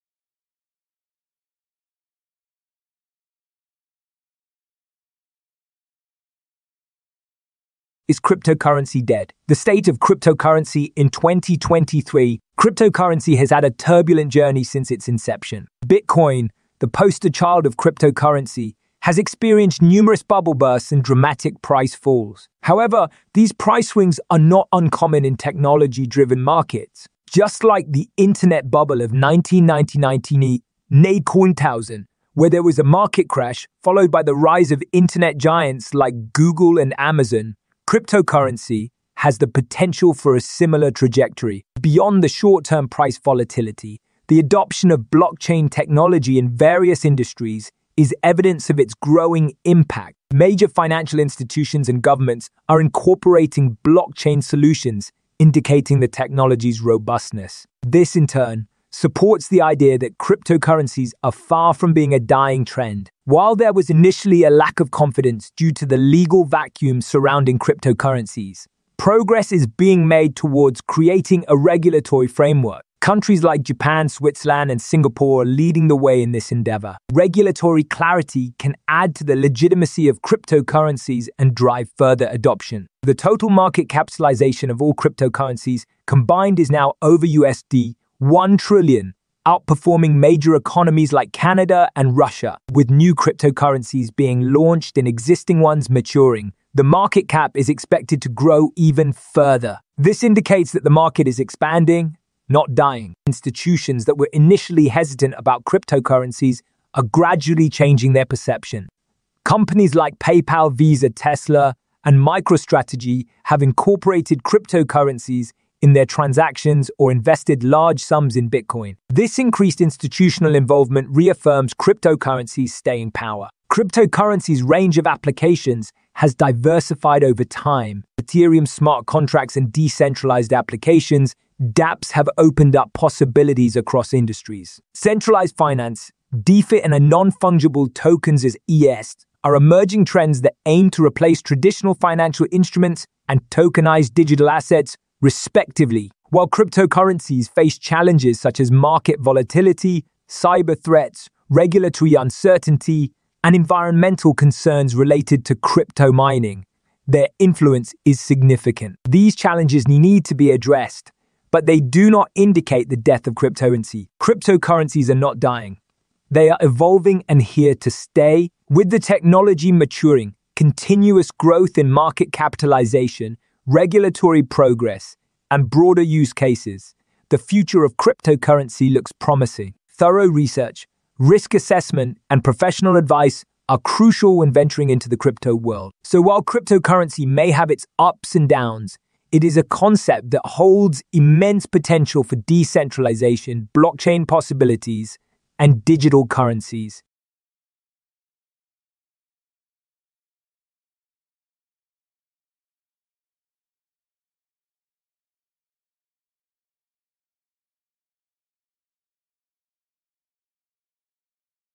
Is Cryptocurrency Dead The State of Cryptocurrency in 2023
Promises of a decentralized financial future, innovative blockchain technology, and astronomical returns are juxtaposed against stories of cryptocurrency crashes, volatile markets, and cyber-security threats. With such contradictory reports surrounding cryptocurrency, the question has inevitably arisen among investors and skeptics alike: Is Cryptocurrency Dead?

This article aims to dissect this multifaceted topic, weigh the evidence from both sides, and provide a well-rounded answer to this pressing question. This examination is crucial, especially now, as the crypto market continues to expand and more investors look towards this high-risk, high-reward asset class.